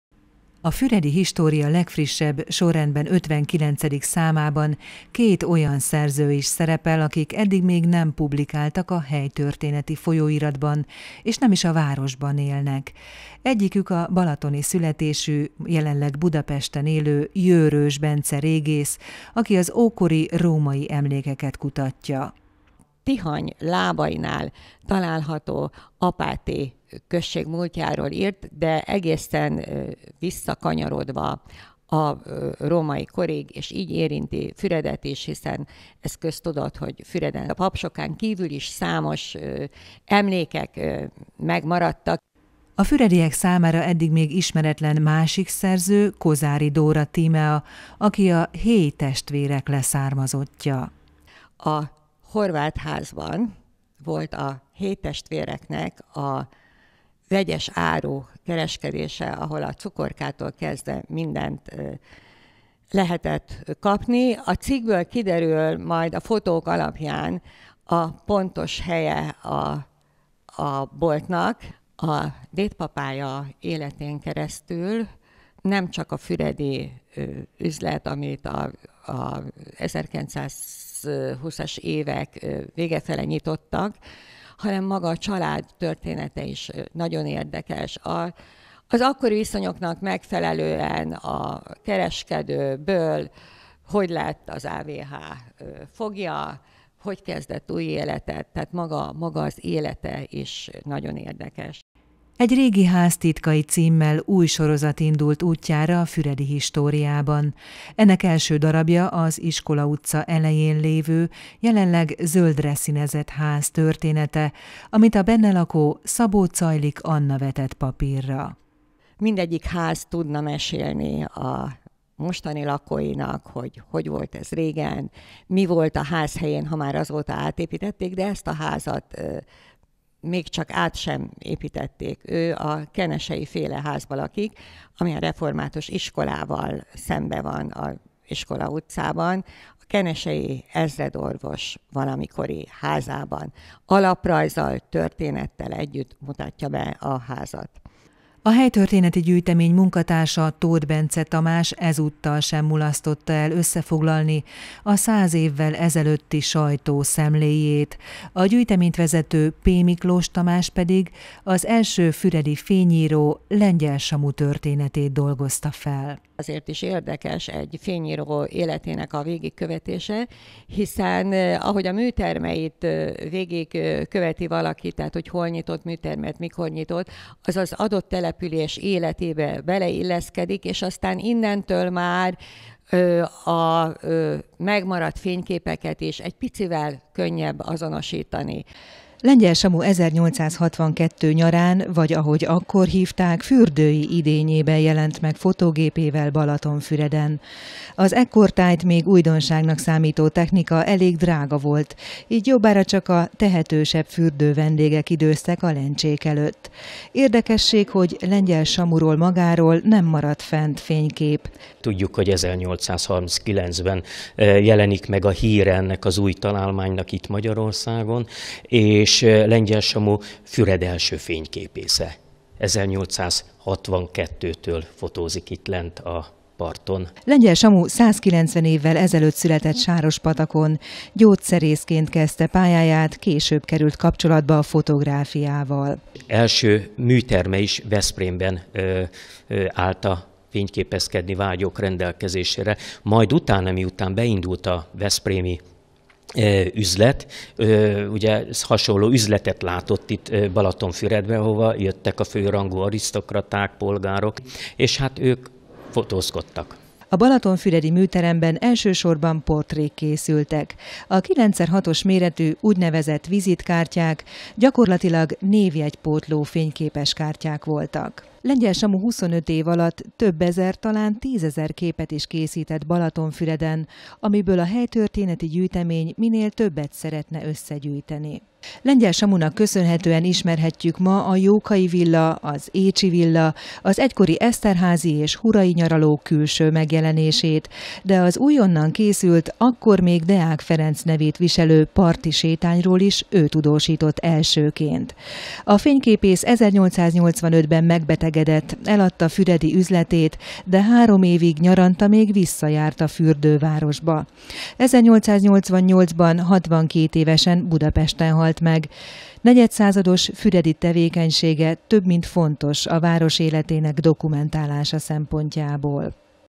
Két új szerző is publikált a Füredi História legfrissebb számában
Apáti középkori falujáról és templomáról, a Héjj-testvérek füredi boltjáról, a város első fényképészéről, Lengyel Samuról és az Iskola utcában lévő Kenessey-ház történetéről is olvashatunk a Füredi História legfrissebb, 59. számában. A nyilvános lapszámbemutató ezúttal nem vált valóra, de az újság a szokott helyeken most is megvásárolható.